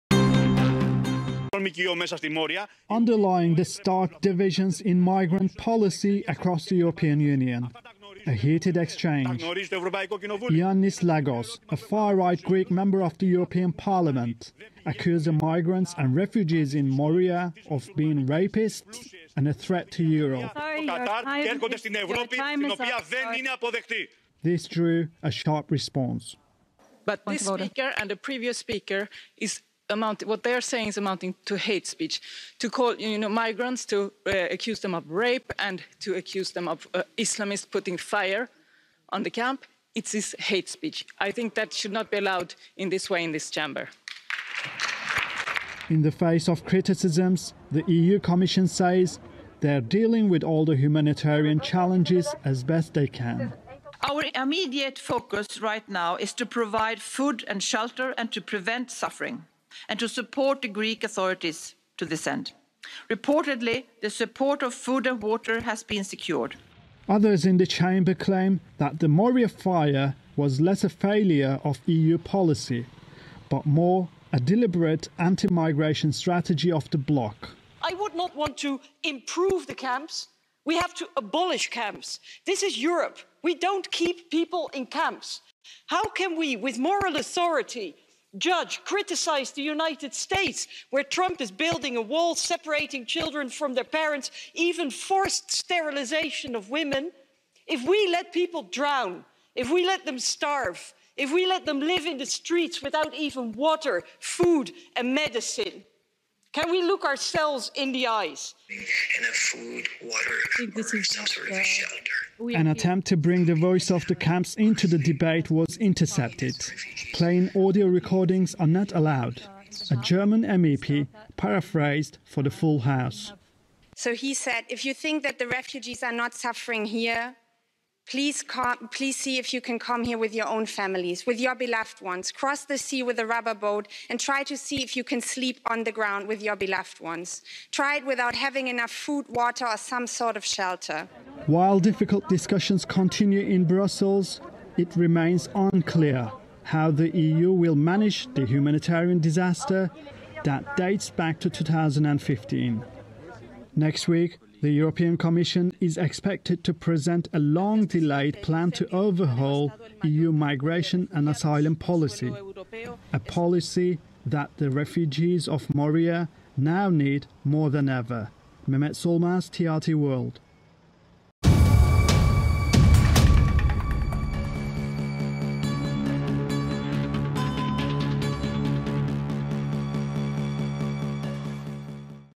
Refugees relocated to new site after Moria camp fire
Police have launched an operation to relocate thousands of refugees who have been sleeping on the streets for over a week since the Moria camp was destroyed by fire. Some refugees are reluctant to move to a new camp that's been built, because of concern over poor conditions. And in Brussels, members of the European Parliament debated the next steps for the Greek island that continues to host the largest refugee camp in Europe. Mehmet Solmaz has more.